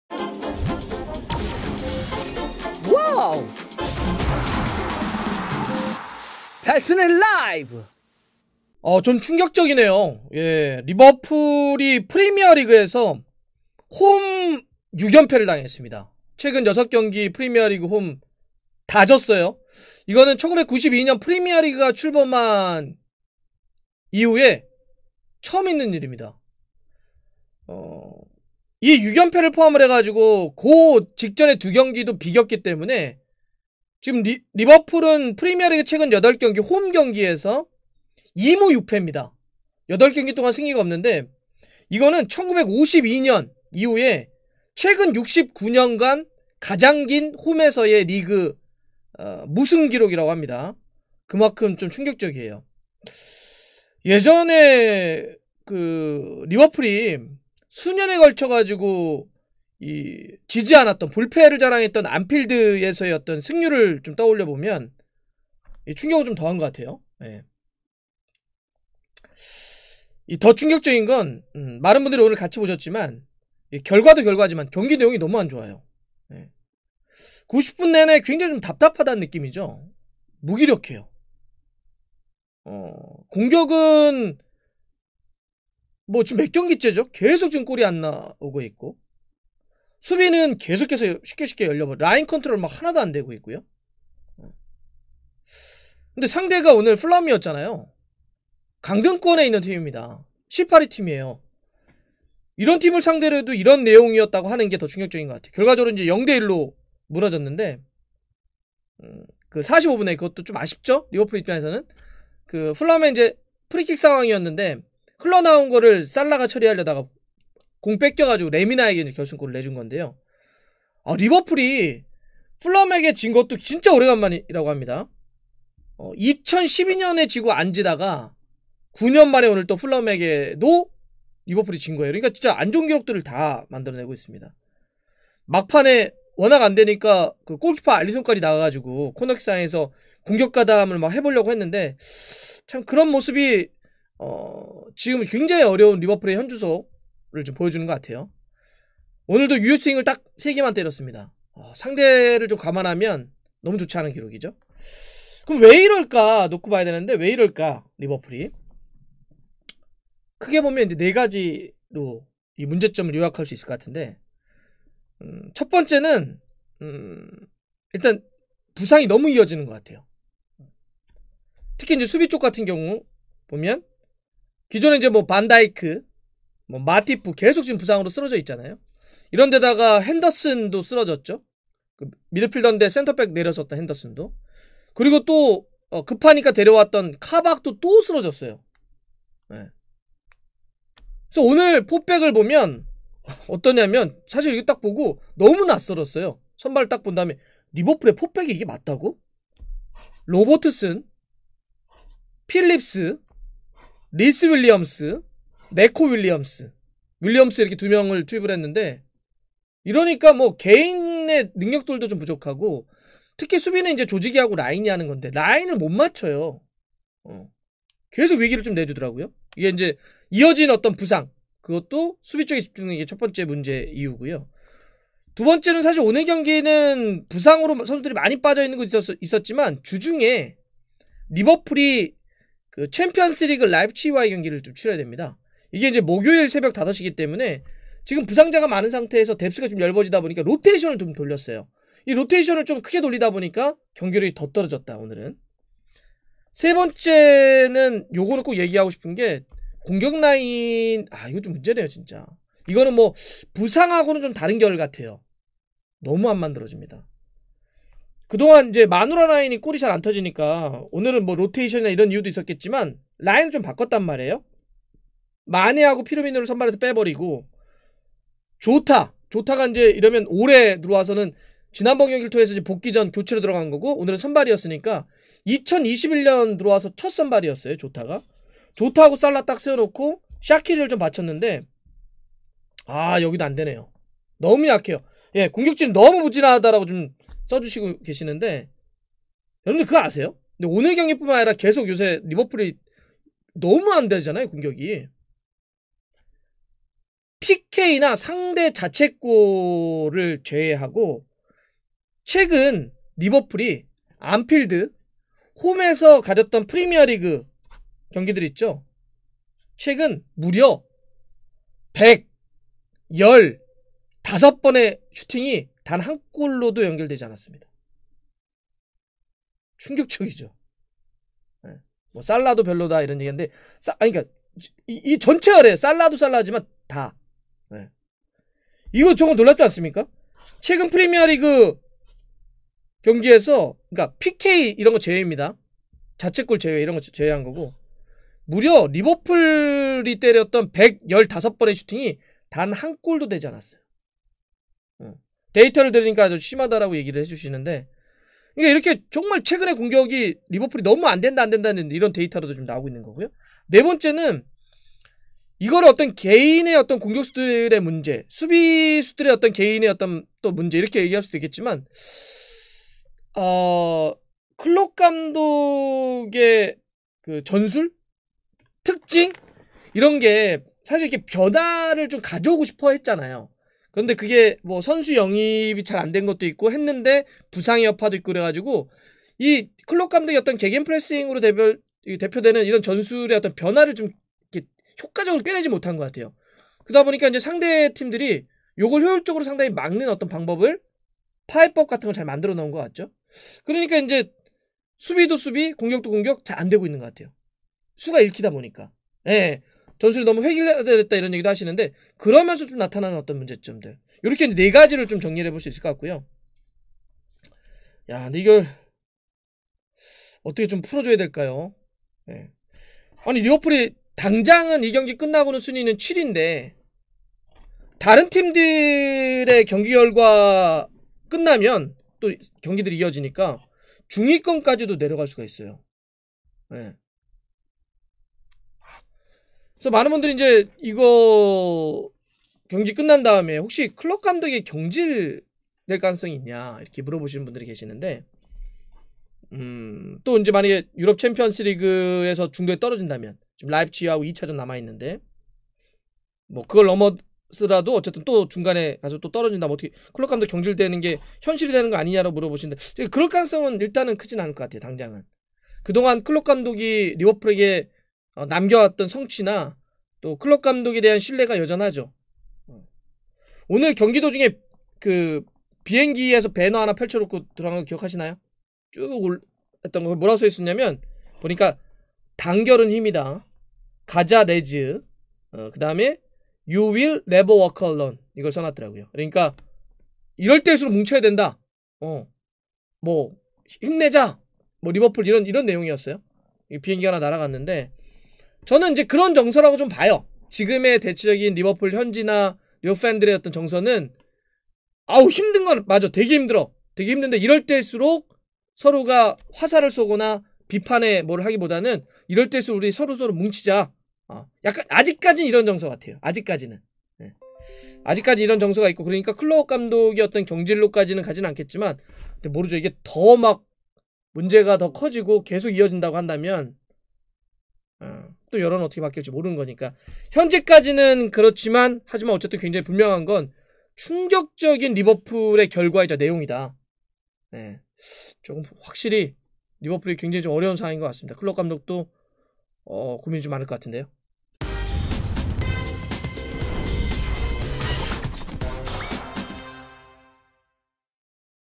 [후토크] 69년 최악 리버풀, 클롭 경질 가능성? 비행기 배너 뭐라 썼나 봤더니 [리버풀vs풀럼]
어?? 이거 리버풀 맞아요??